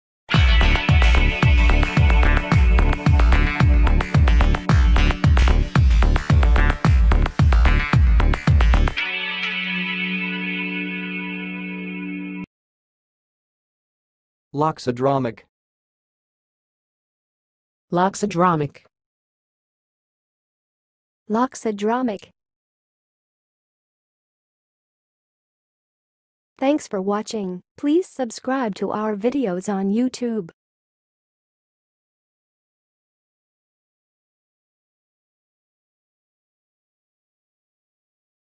How to say "loxodromic"! (High Quality Voices)
Watch in this video how to say and pronounce "loxodromic"!